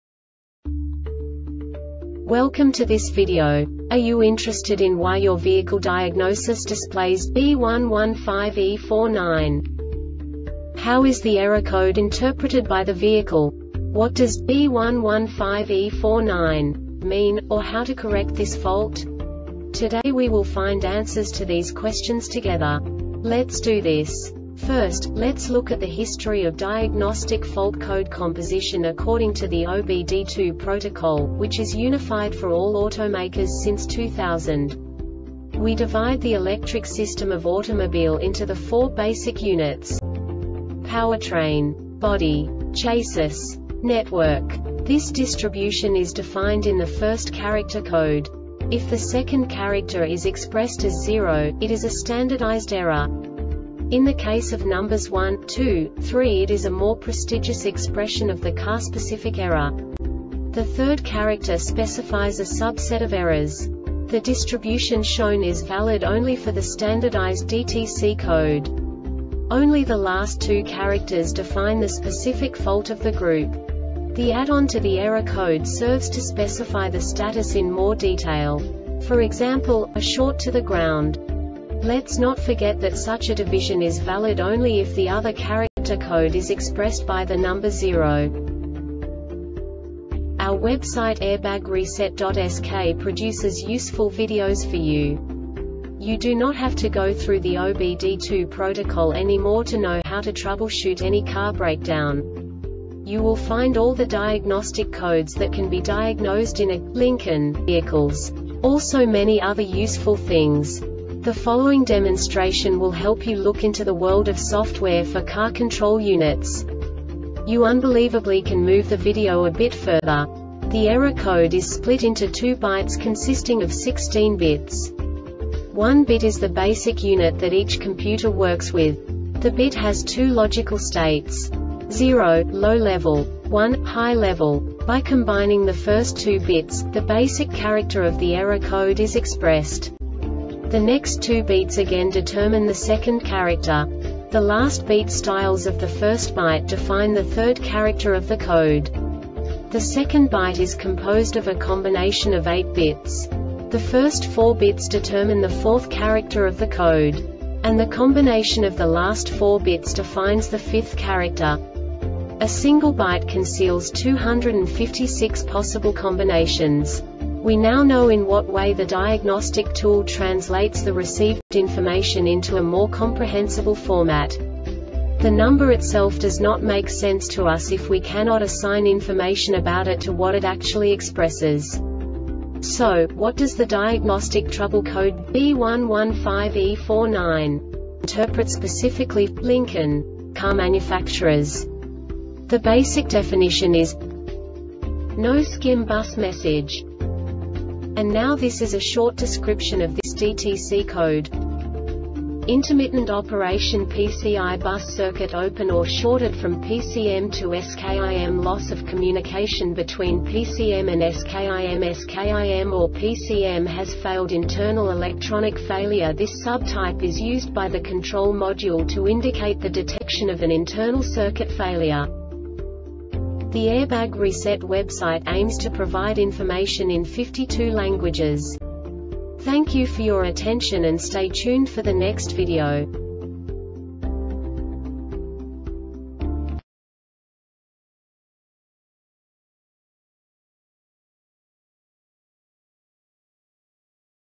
DTC Lincoln B115E-49 Short Explanation
Contents:
0:21 Basic DTC analysis according to OBD2 protocol standard.
1:48 Insight into programming
2:58 A diagnostically understandable description of the Diagnostic Trouble Code
3:05 Basic error definition

Make:
Lincoln
Code:
B115E 49
Definition:
No SKIM Bus Message
Cause: